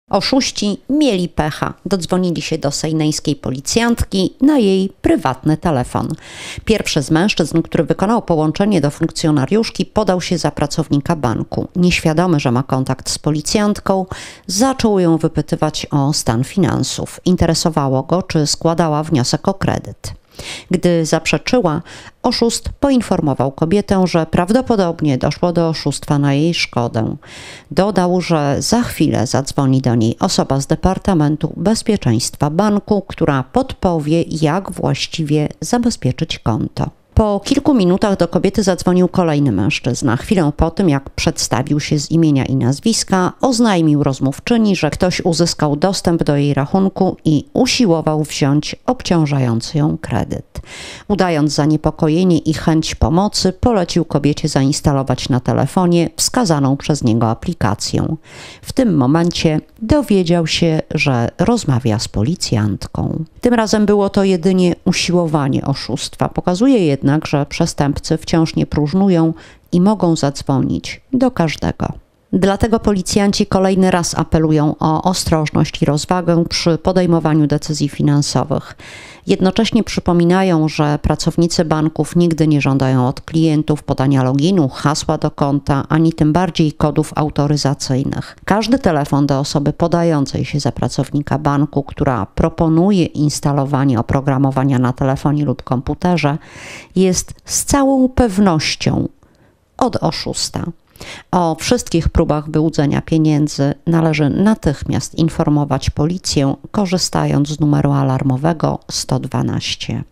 Sejny: trafiła kosa na kamień! |TVSUWAŁKI
Oszuści mieli pecha. Dodzwonili się do sejneńskiej policjantki, na jej prywatny telefon.